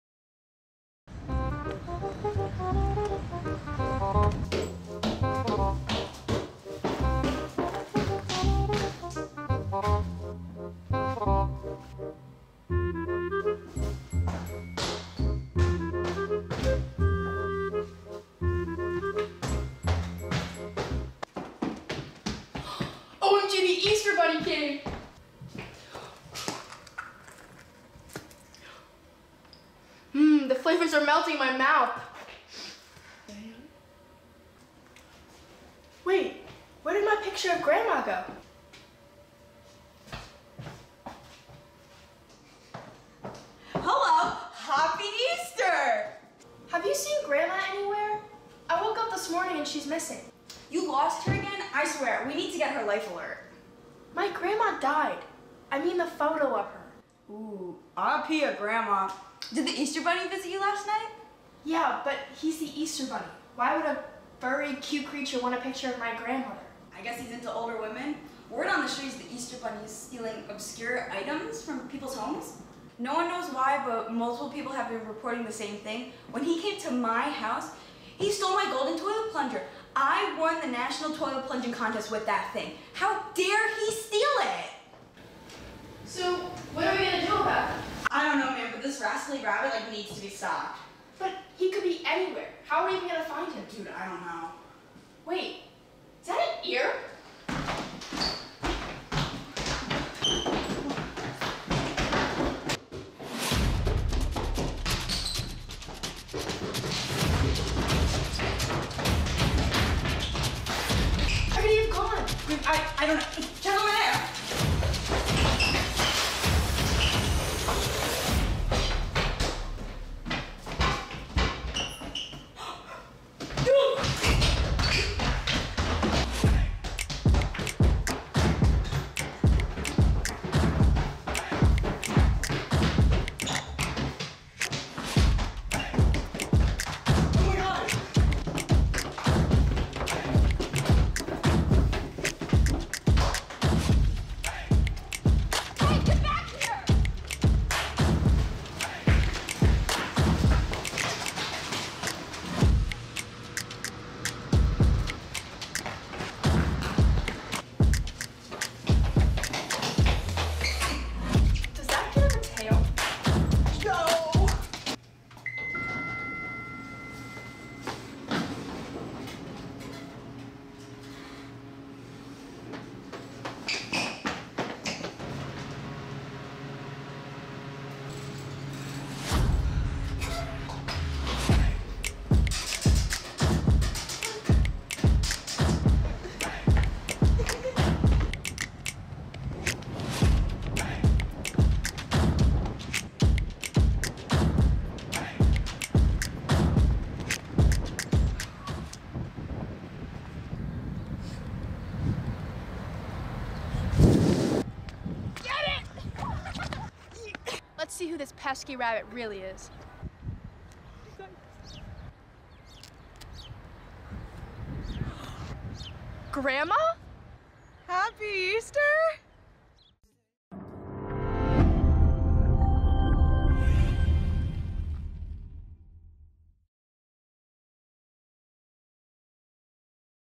Thieving Easter Bunny
“You lost her again? I swear we need to get her a life alert.”

“Jordan… my grandma died, I mean the photo of her.”

EXECUTIVE PRODUCERS:
Faith Bartell
JT Maher
Kate Zincone

WRITER:
Alli Brown

DIRECTOR:
Johnny Wasdin

ACTORS:
Lauren Cap
Maddy Troutman
Alli Brown